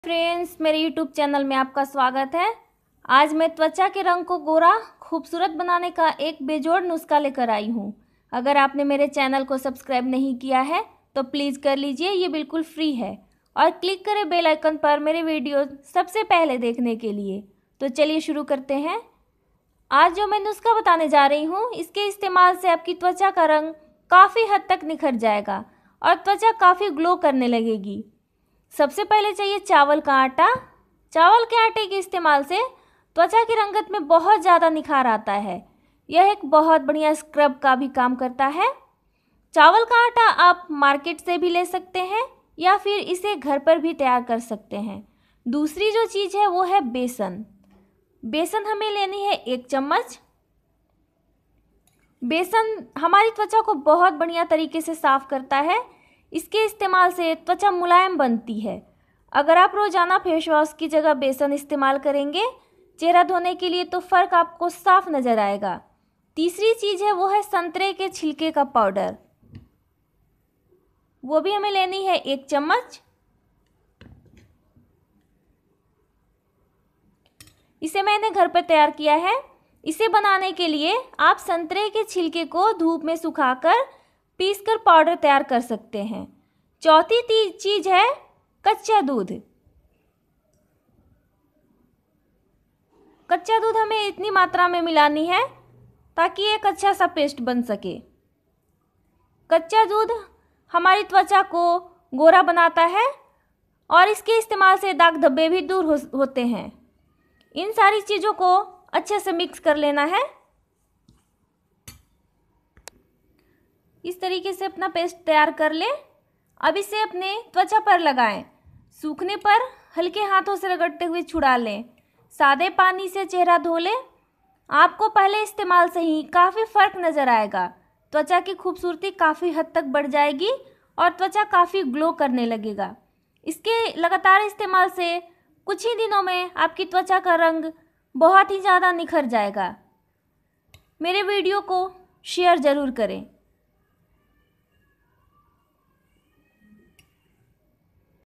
इस चमत्कारी नुस्खे से त्वचा इतनी ज्यादा गोरी हो जाएगी की यकीन नही होगा!!
चावल का ये नुस्खा आपकी त्वचा को इतना गोरा कर देगा की देखकर हैरान रह जाएंगे!!